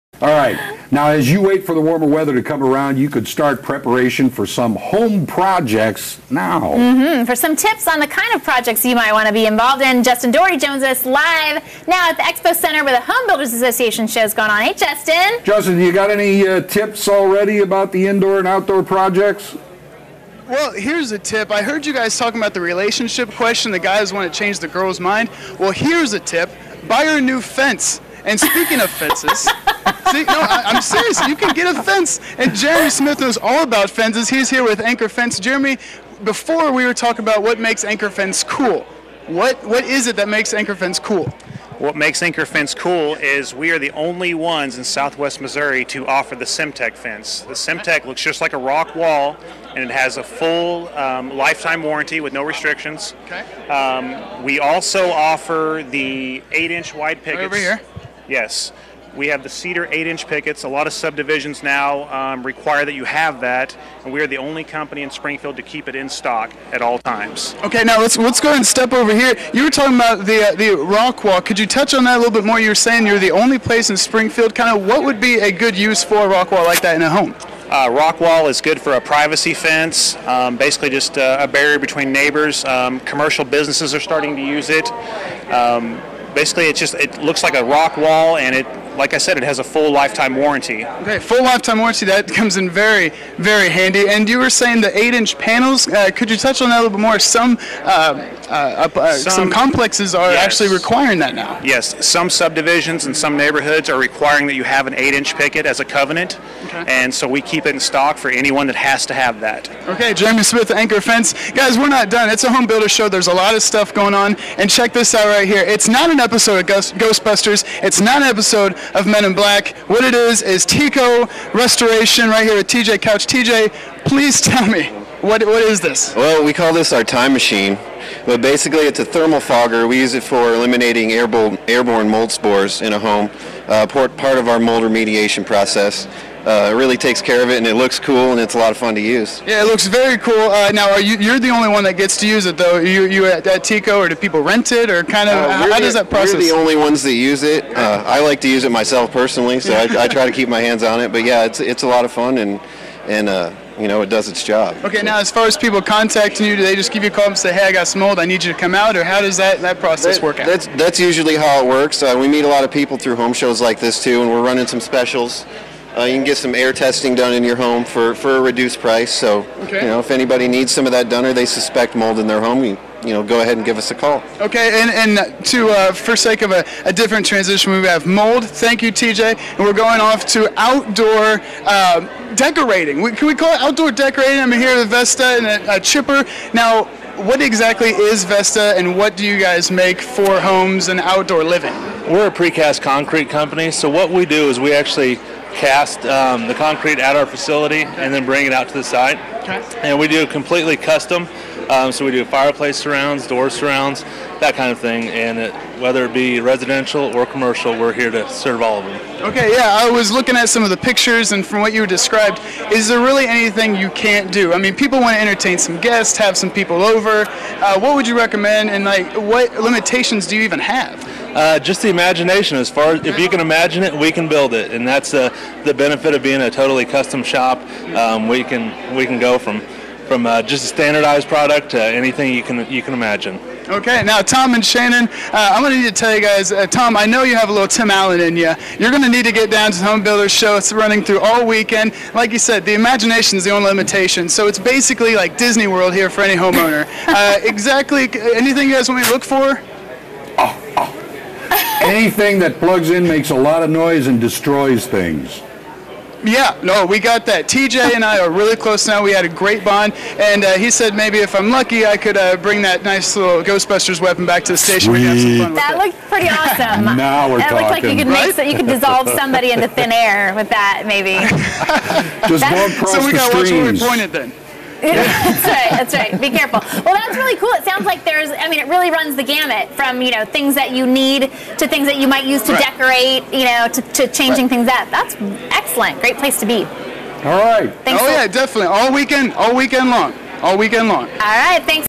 Justin Dougherty Live at the 2013 HBA Home Show
Justin Dougherty reporting live with three interviews at the 2013 HBA Home Show. Anchor Fence, TECO Restoration and Vesta Precast Accents. The 2013 HBA Home Show was January 18, 19 and 20.